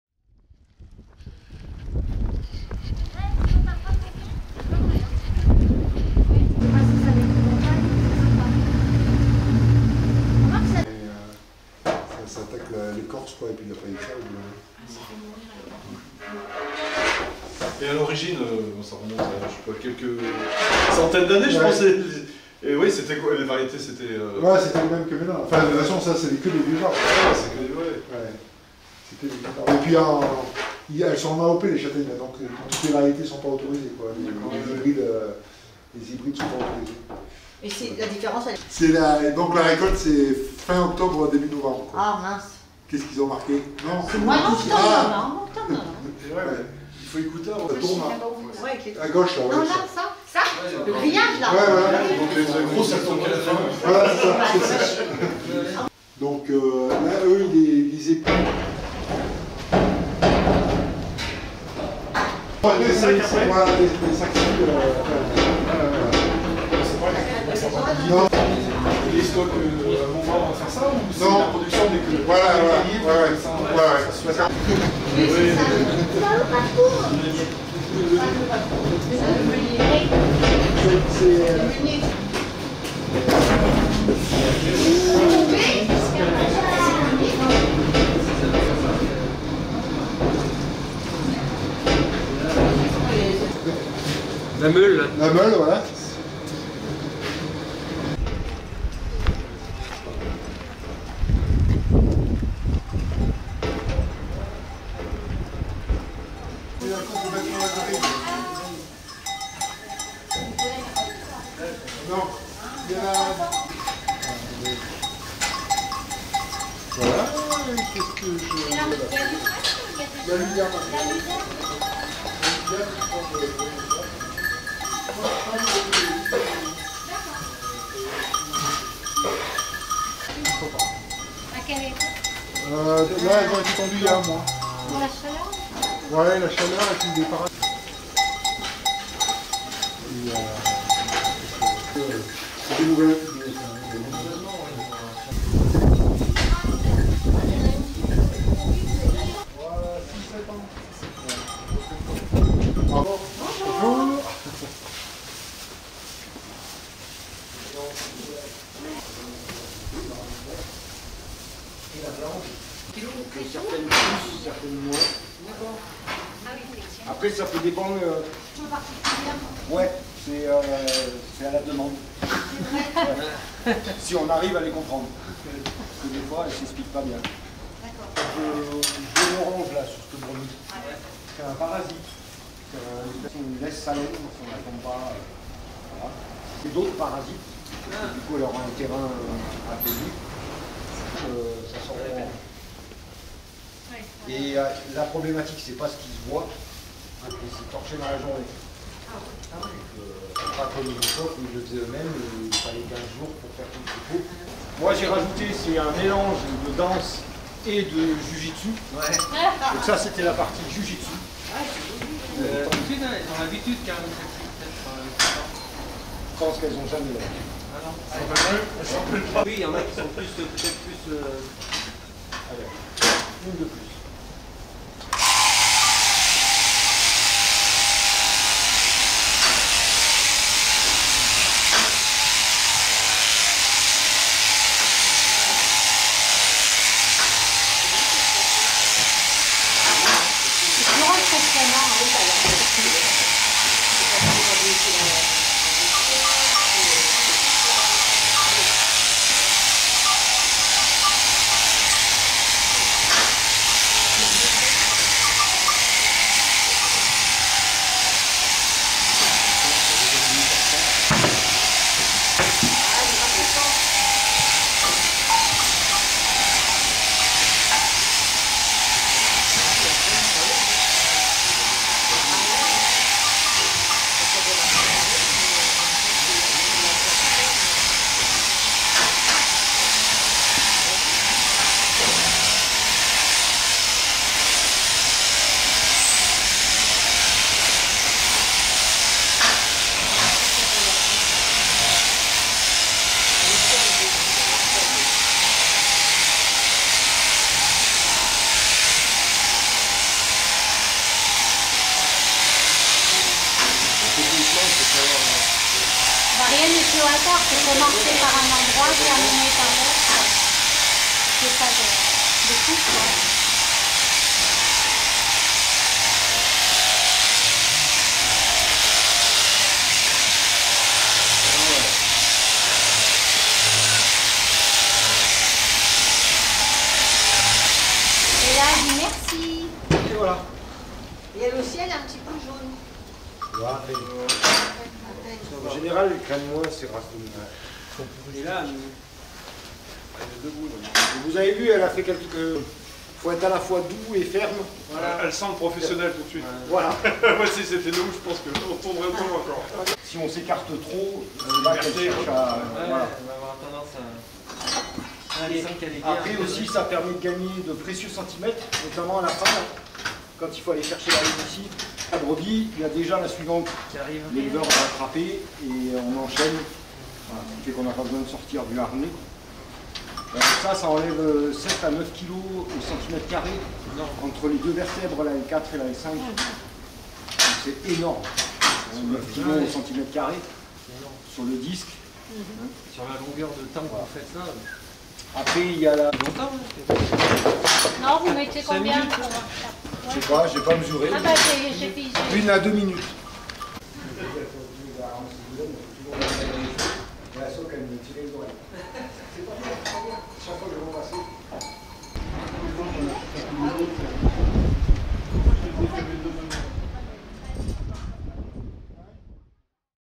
GAEC de Samarie - Châtaigne et brebis (Saint-Jean-Chambre - Ardèche - France)
Visite pendant « De ferme en ferme 2024 »